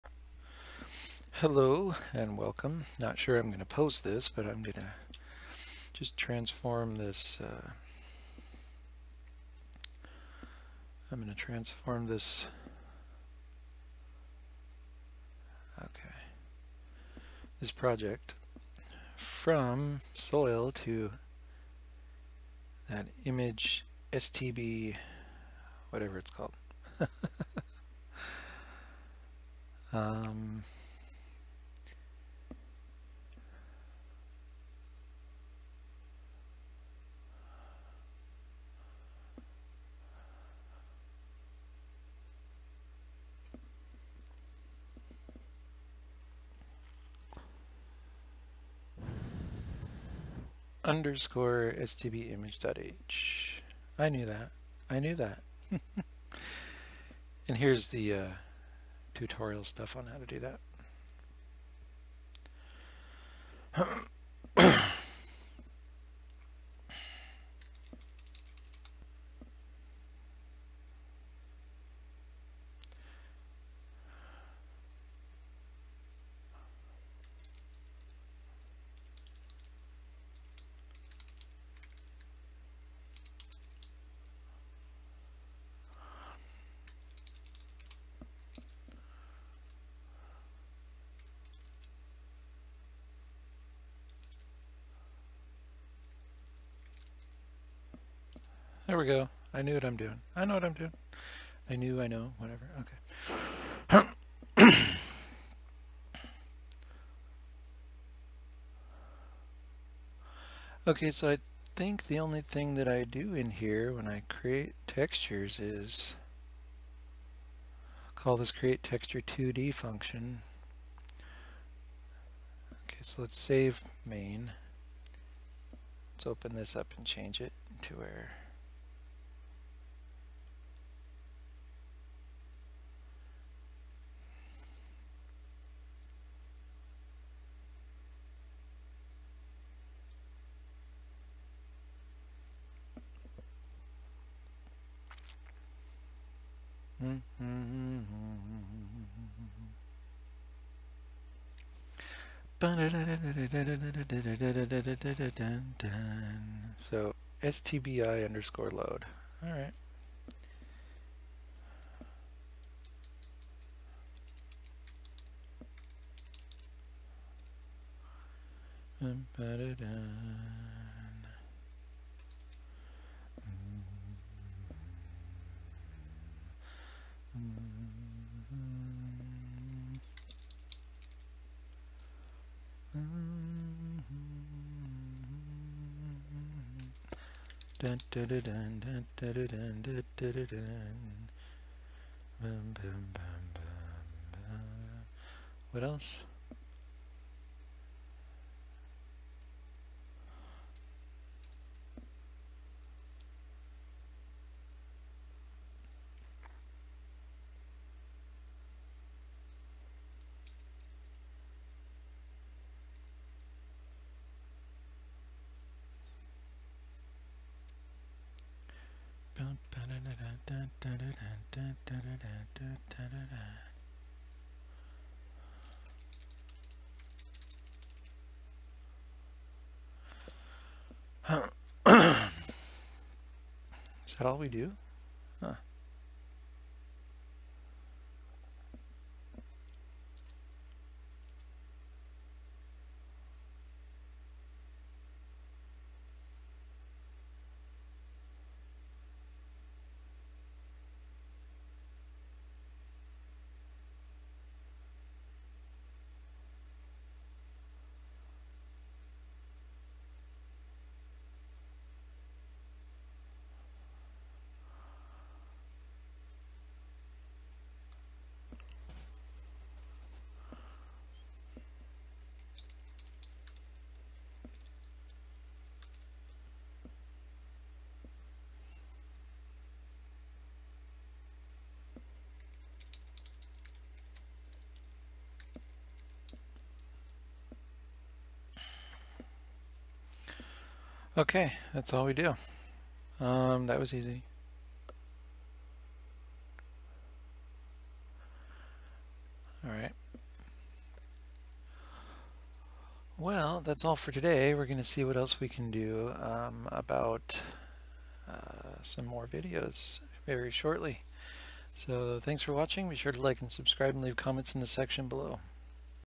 OpenGL LearnOpenGL COM Converting a SOIL project to STB IMAGE H Visual Studio 2017
I just did a quick conversion from SOIL to stb_image.h for this project, so I thought it would make a nice video to add to the list.